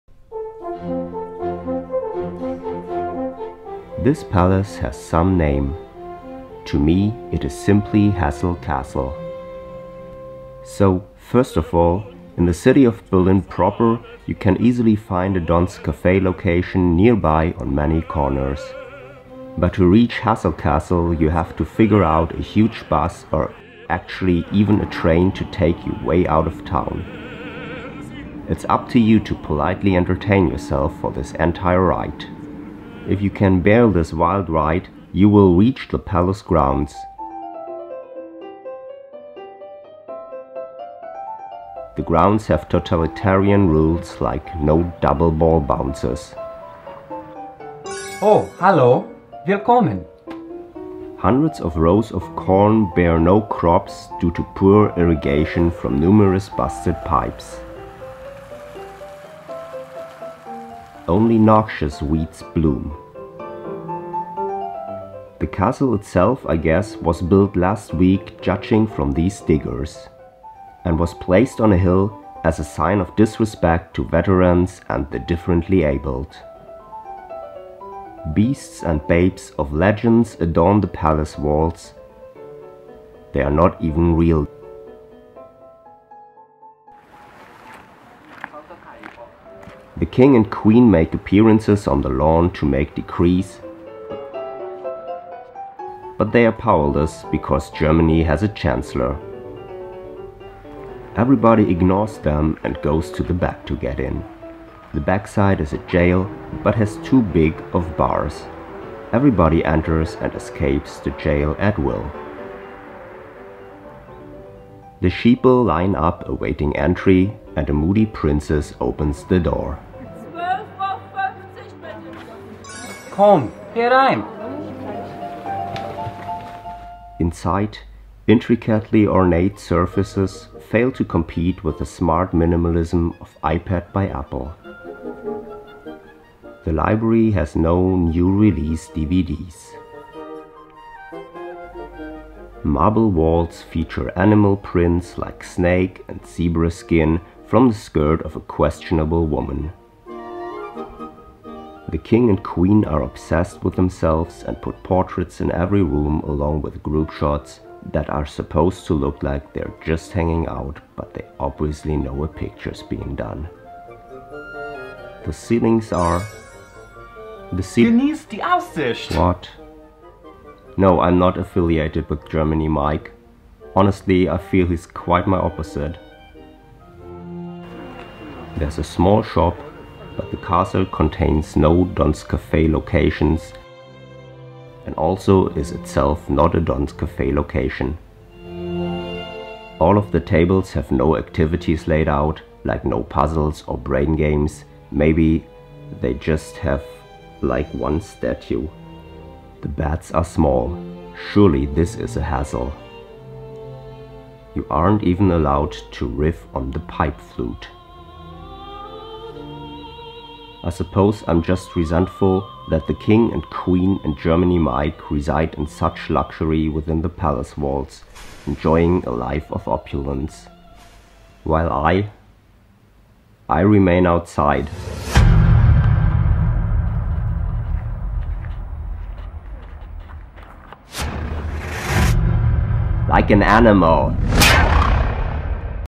A Tour of the Castle (Eine Tour durch das Schloss)
This video is a leisurely-paced exploration of an uncool and unfair palace. Don’t even try to turn the subtitles off.

Narrator:  Dirk Mahlmeister
Germany Mike:  Robert Woods

German Translation by Dirk Mahlmeister

Everything else by Spencer Higginson

Special thanks to Margaret Higginson for helping out, and to Laurie and Charles Higginson for paying the €3 photography fee.